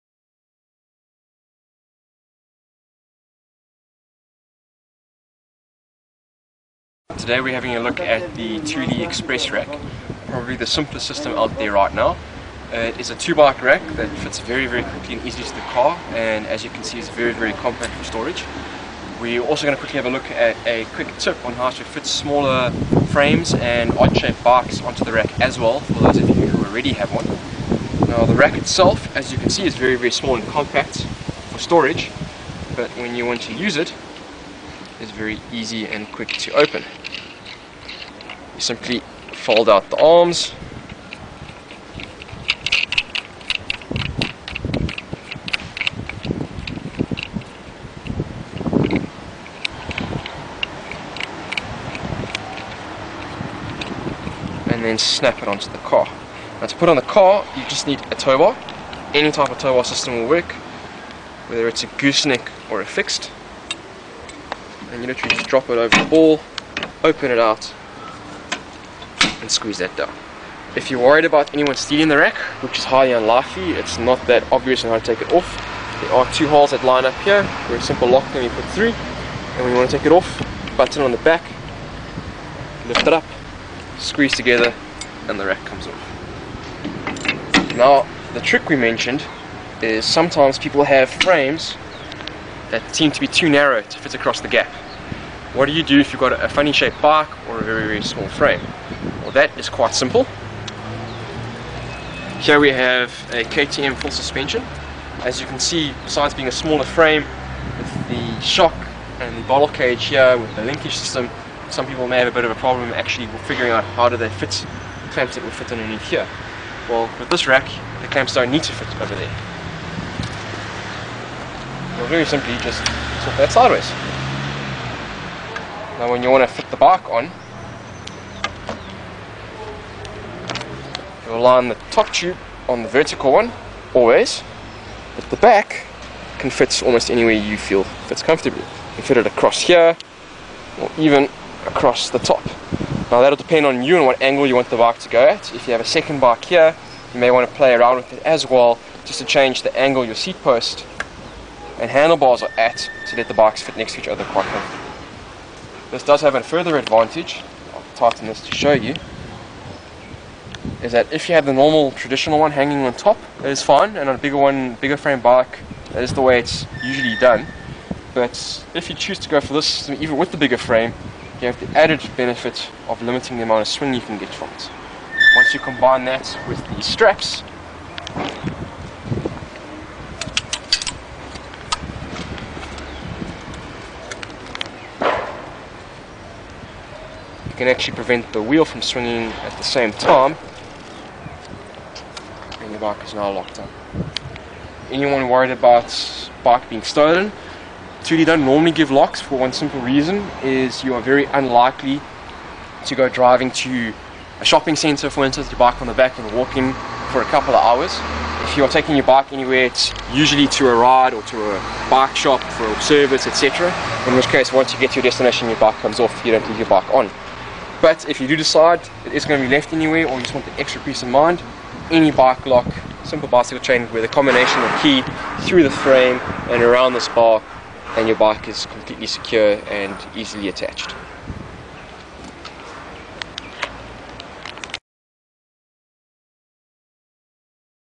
Cycle Lab - Thule Xpress 2 bike carrier
The NEW Thule Xpress 970 is probably the quickest and simplest cycle carrier to fit currently on the market today. Fitment takes a matter of seconds and it would be ideal for you if you regularly carry 1 or 2 bikes and have a towbar fitted to your vehicle.

The 970 - Xpress 2 Bike Tow Bar Mounted Carrier is a lightweight tow bar mounted carrier which is designed to carry up to 2 bikes and offers rubber coated frame holders which keep the bikes firmly in place while protecting them from damage. The 970 - Xpress 2 Bike Tow Bar Mounted Carrier also folds flat when not in use for easy storage and is even compact enough to store in the trunk of a car.

Features
• Rubber coated frame holders keep the bikes firmly in place while protecting the frames from scratches and marks.
• Relfexes on the endcaps provide extra safety in traffic.
• Folds flat for easy storage, compact enough to be kept in the trunk of the car.